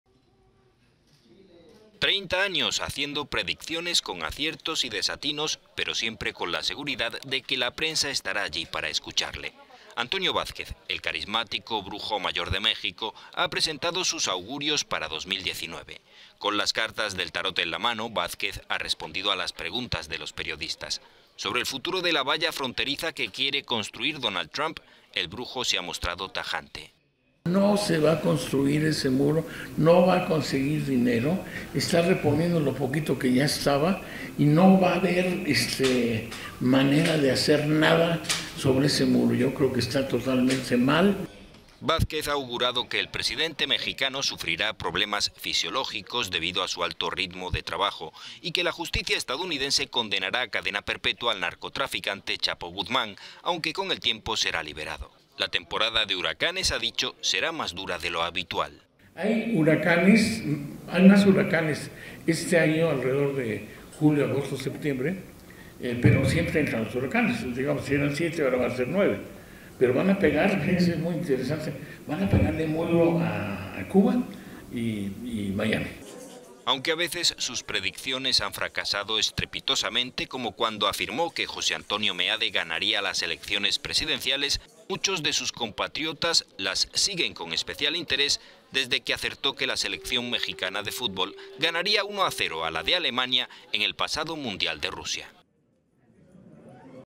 El "brujo mayor" de México augura que Trump se estampará contra el muro fronterizo
Donald Trump se estampará contra el muro fronterizo. Son los augurios del "brujo mayor de México", Antonio Vázquez, que acaban de presentar sus previsiones para el año 2019. …